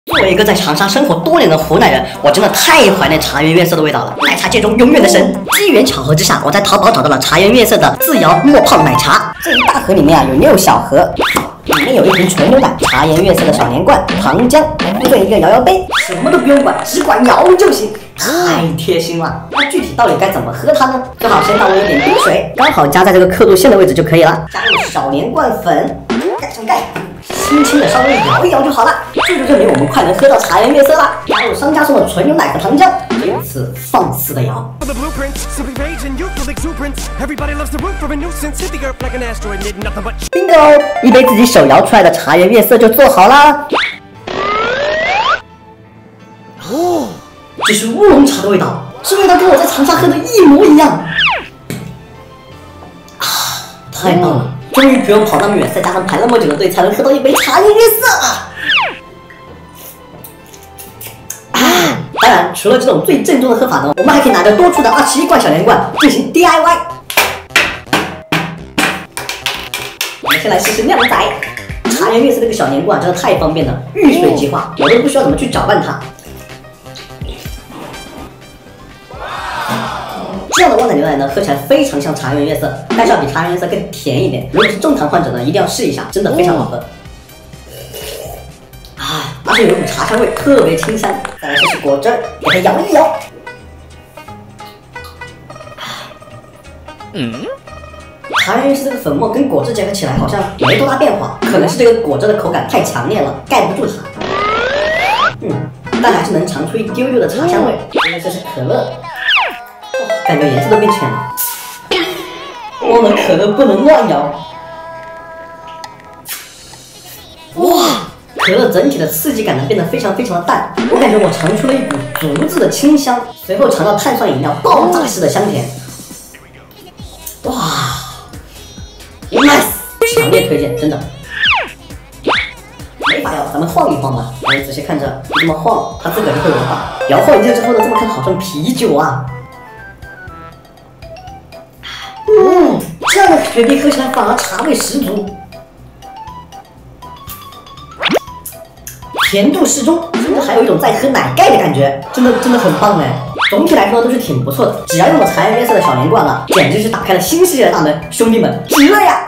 【架麽饞】在家就能做茶顏悅色奶茶，搖一搖就能出泡沫，和買的一樣好喝！
小陽真的太懷念茶顏悅色的味道了，奶茶界中永遠的神，於是在網上找到了自搖沫泡奶茶，味道一模一樣，真是愛了愛了~

大家好，這裡是【架麼饞】
這裡每天會和大家分享奇特有趣的美食。